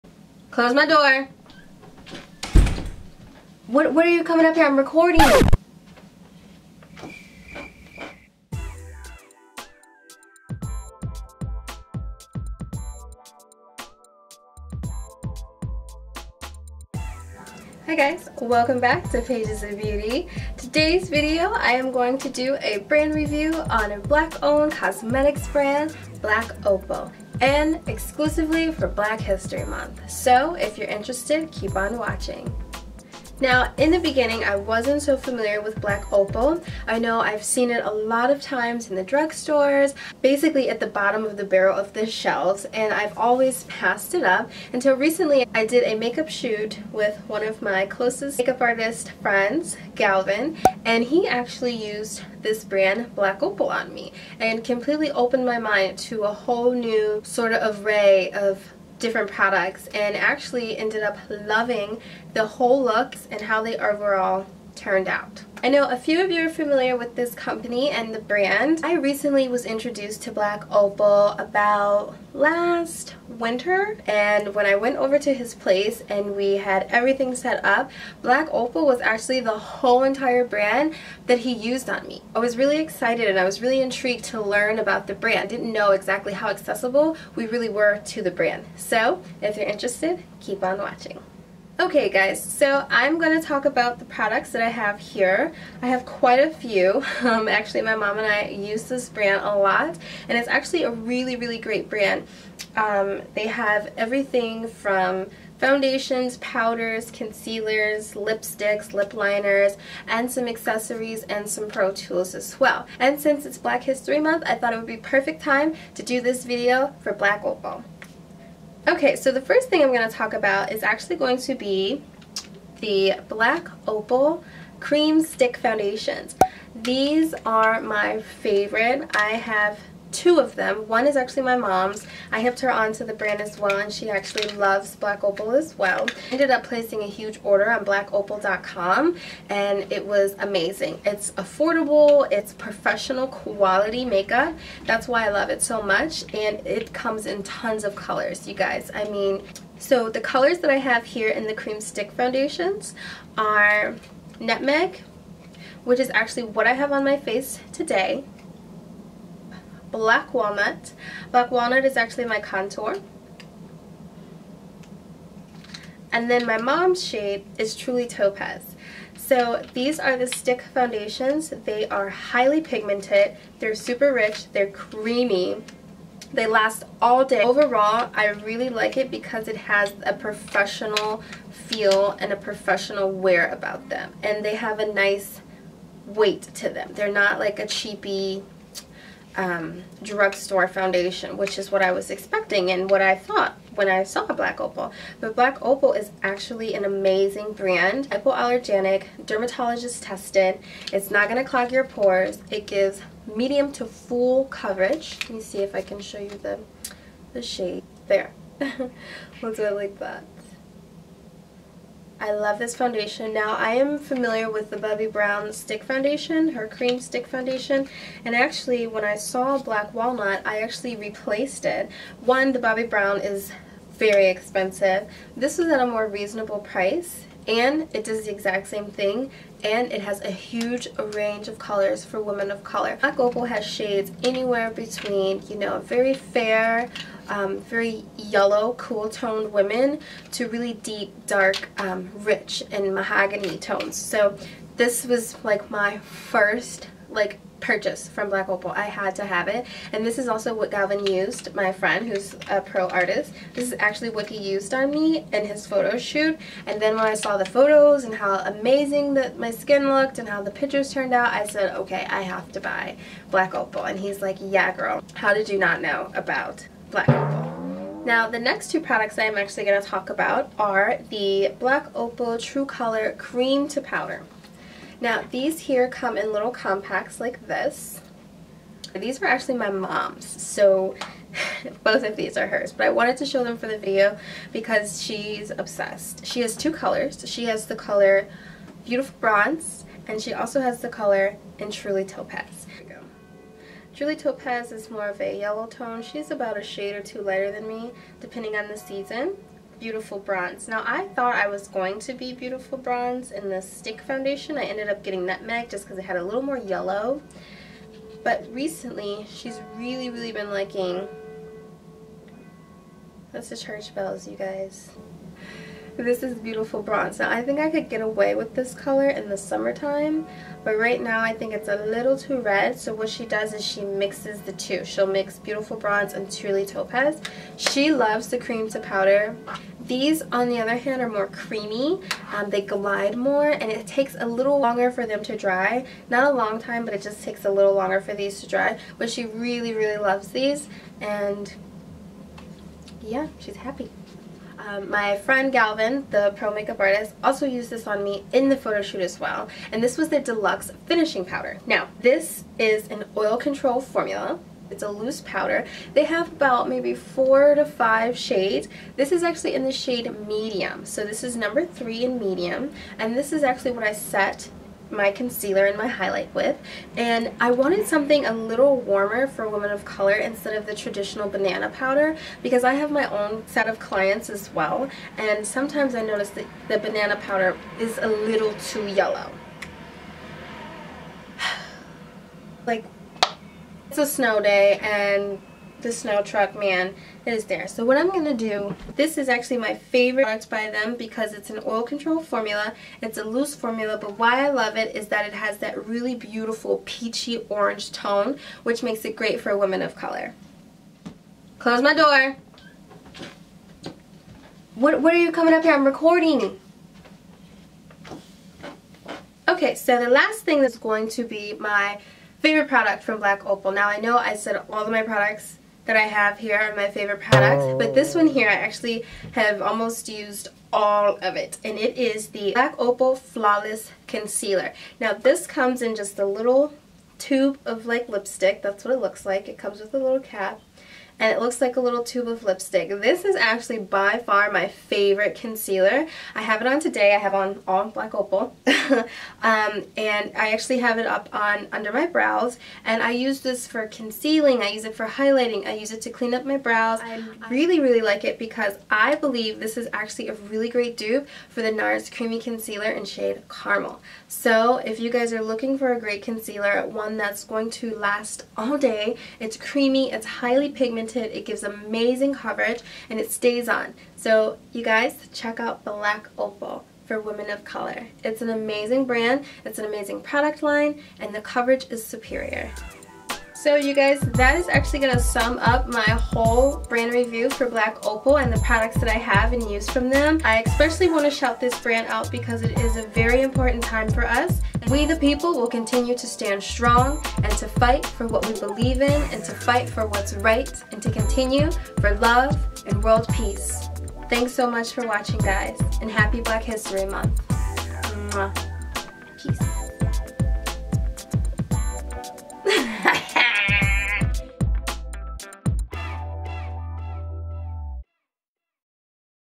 Black Opal for Black History Month | Brand Review
Today I am going to be covering Black Opal Cosmetic's for Black History Month!!

I'm so excited to upload this video for all my sister's and I hope you find this video informing.
Especially for the women of color who have a hard time finding the perfect FOUNDATION SHADE!
Look no further Black Opal has our backs! I hope you all enjoy the video and check out this STRONG GROWING BEAUTIFUL BRAND!
xoxox
-Paige

Products Mentioned |

Black Opal True Color Stick Foundations Nutmeg, Truly Topaz, Black Walnut -

Black Opal True Color Creme to Powder Foundation Truly Topaz, Beautiful Bronze

Say it Loud!